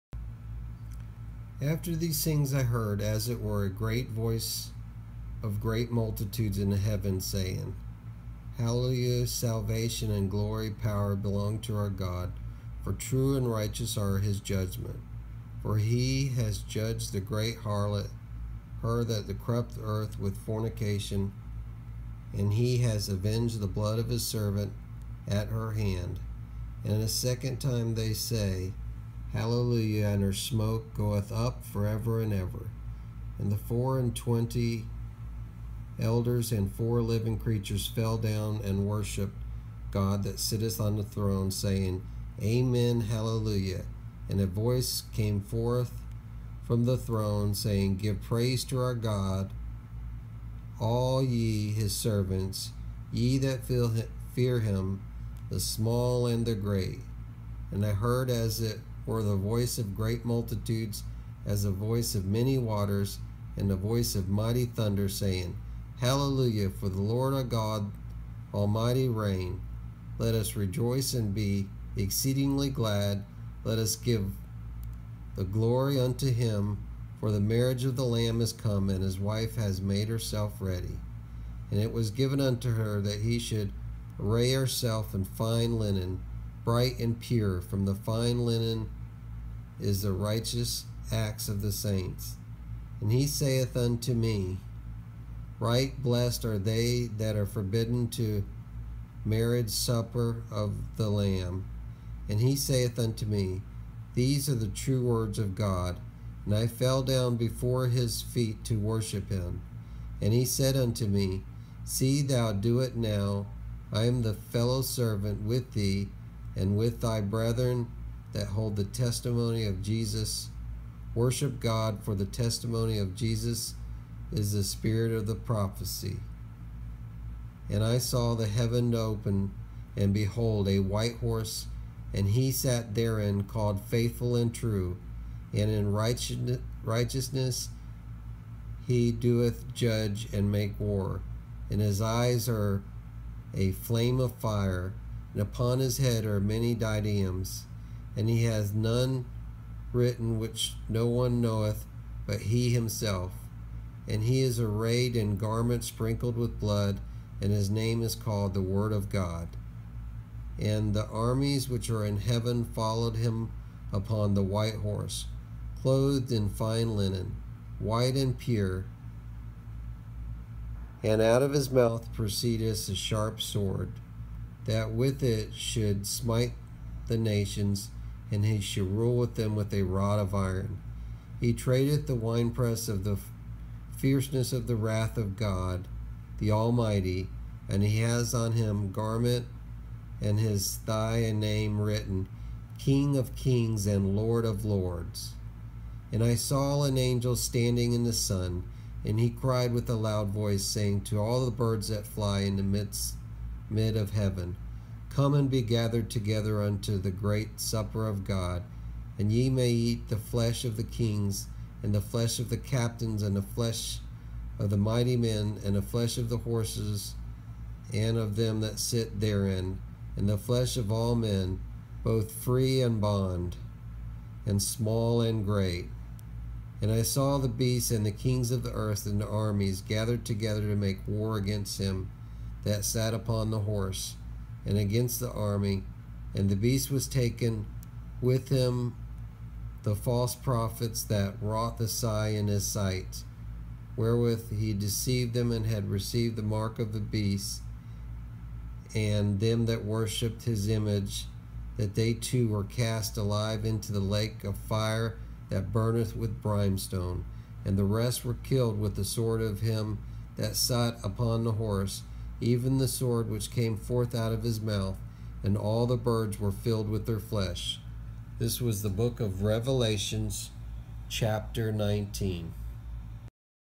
ASV 1901 Revelation 19
Daily Bible Reading- American Standard 1901- Revelation chapter 19
Savannah Ga